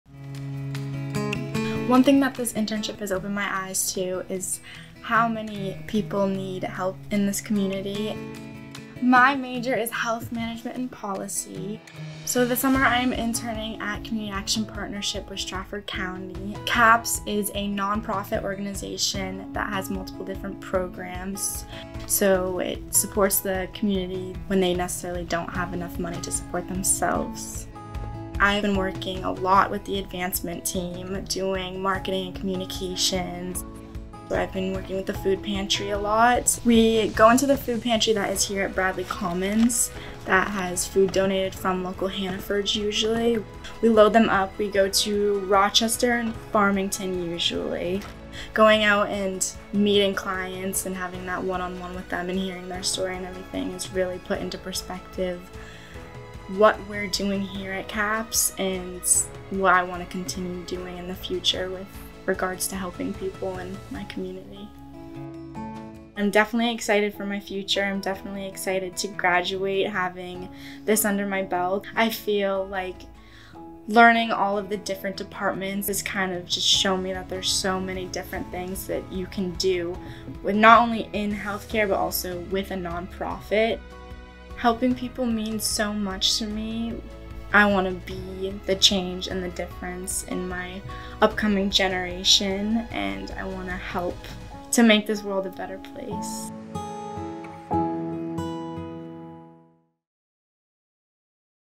UNH Voices of Wildcats: Sophia Bolduc '23
Sophia Bolduc ‘23 experienced an eye-opening summer internship at a Strafford County nonprofit. Her work there revealed both how she can help her community and her many career options after graduating with her Health Management and Policy degree from UNH.